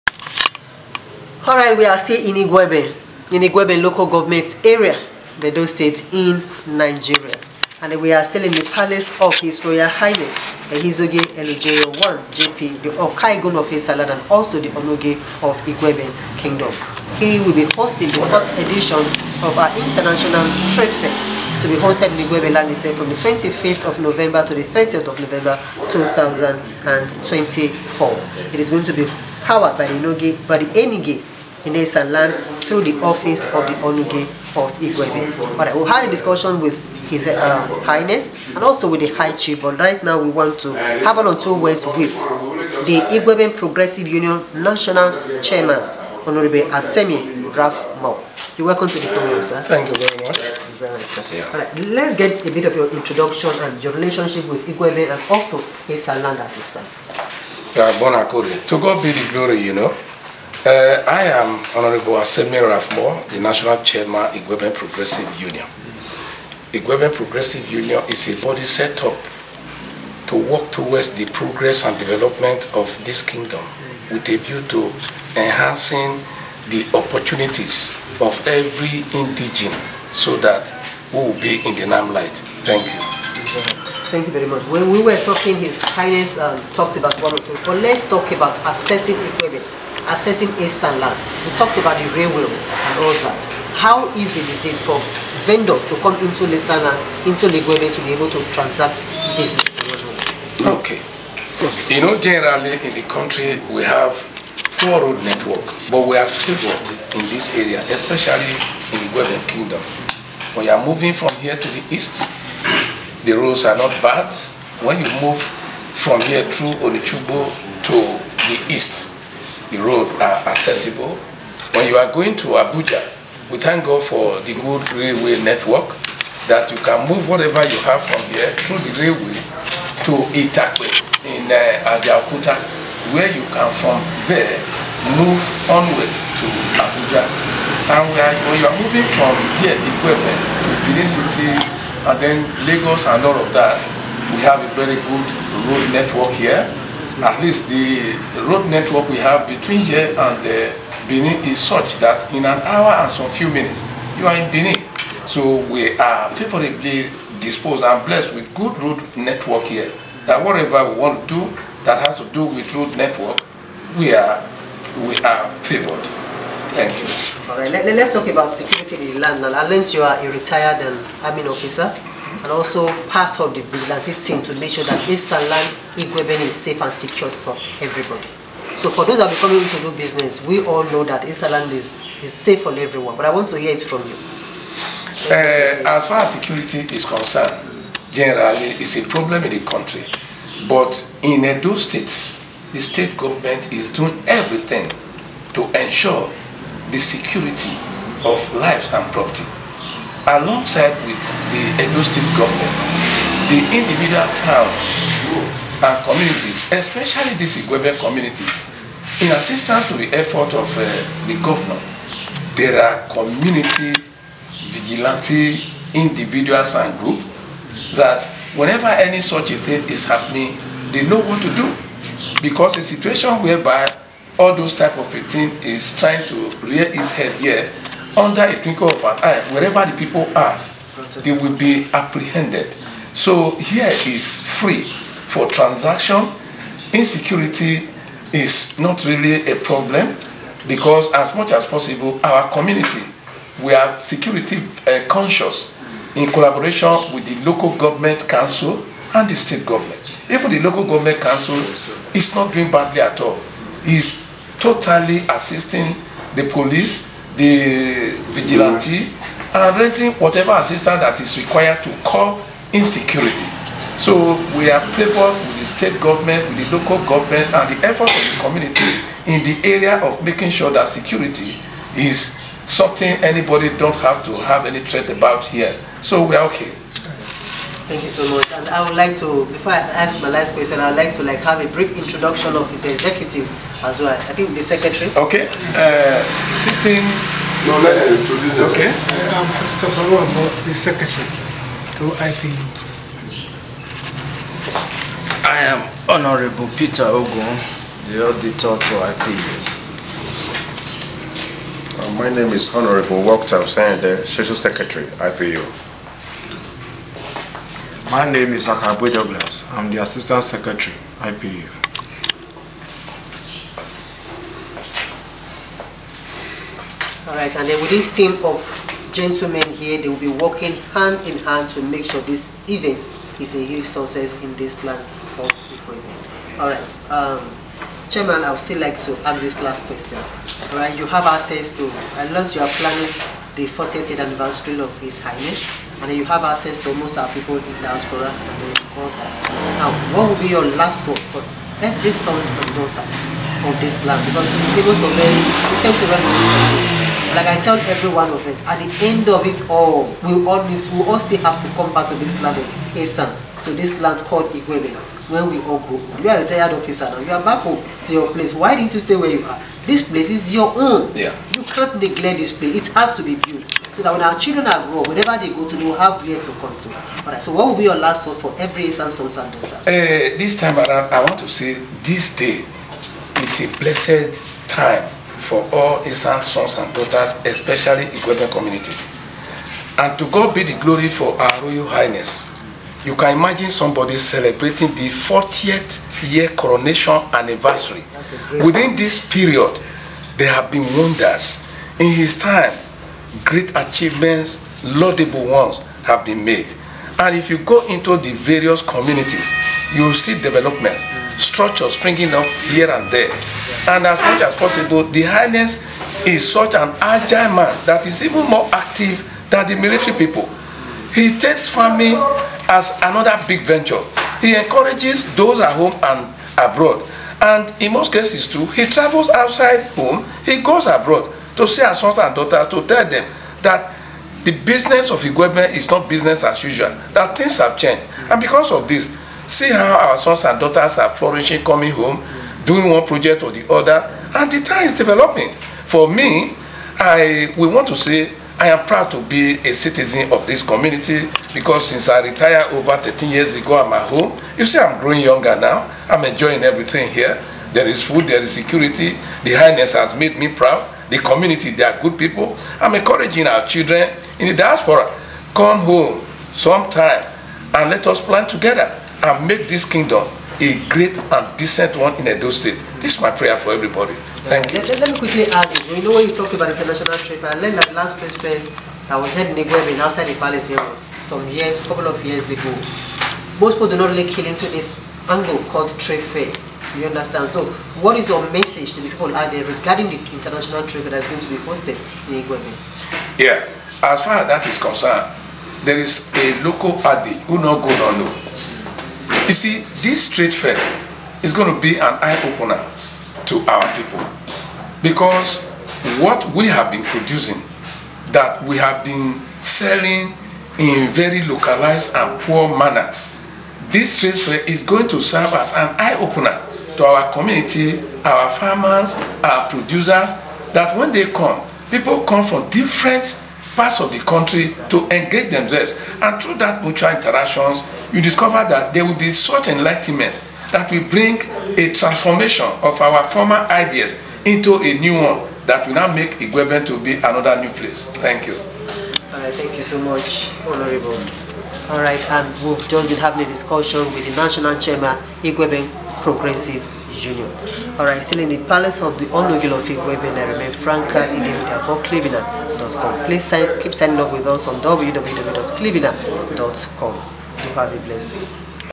Igueben Progressive Union with COP Seminar and International Trade Fair 2024
Hon.  RAPHMOORE ASEMIEN.
The National Chairman, Igueben Progressive Union, Igueben Kingdom, Edo State, Nigeria

Clevenard Open Project (COP)Seminar and International Trade Fair 2024: Empowering Minds and Boosting the Local Economy of Esan Land

Event Dates: Monday 25th  Till Saturday 30th of November 2024
Venue: Igueben Town Hall, Igueben, Edo State, Nigeria, Time: 10:00 AM prompt

POWERED BY THE: Enigie in Esan land, through the office of the Onogie of Igueben and Okaigun of Esan land, HRH Ehizogie Eluojierior I, JP

Esan Land: Be Proud of Your People
Esan People: Be Proud of Your Kings

Phone Numbers:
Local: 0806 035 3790